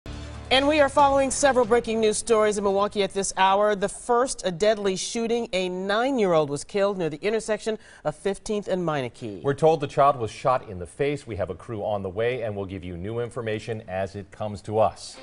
9 year dies in shooting
Child was shot in the face